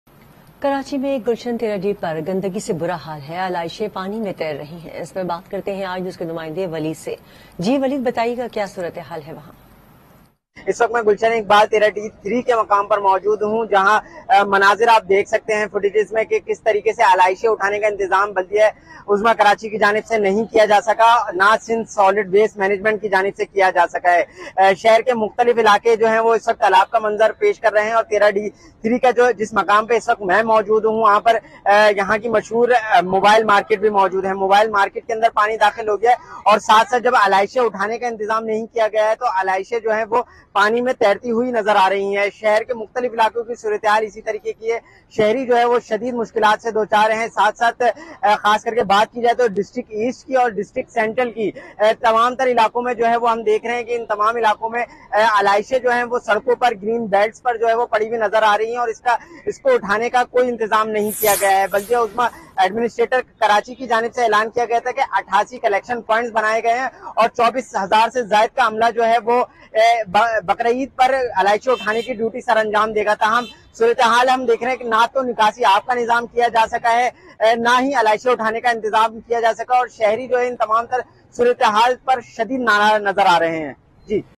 Karachi main beemari phailnay ka khadsha | Qurbani kay janwaron ki bakiyat barish kay pani me shamil
Karachi main beemari phailnay ka khadsha |  Qurbani kay janwaron ki bakiyat barish kay pani me shamil | Aaj News

Aaj TV Official:
shorturl.at/iBDNX